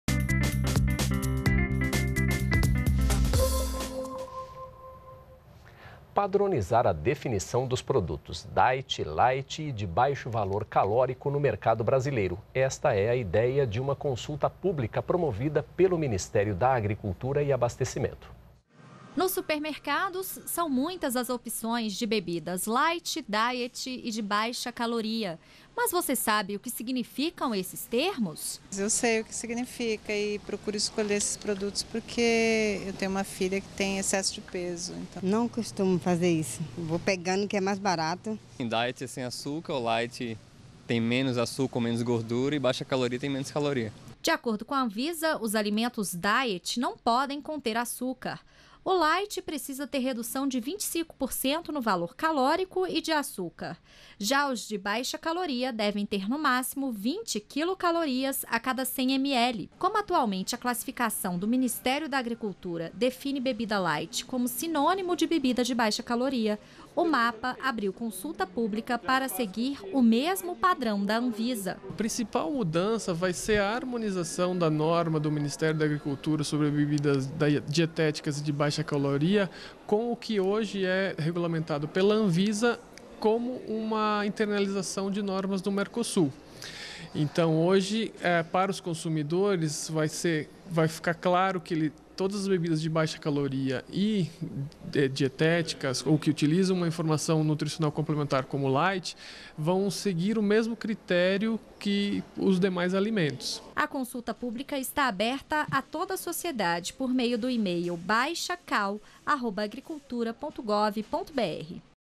Aberta consulta pública sobre bebidas dietéticas e de baixa caloria
NBR NOTÍCIAS - 06.02.15: O Ministério da Agricultura abriu consulta pública para fixação de padrões de identidade e qualidade para a bebida dietética e de baixa caloria. A proposta visa complementar as regras em vigor para esses tipos de bebidas, Os interessados podem enviar sugestões até o dia 5 de março. Saiba mais em: agricultura.gov.br.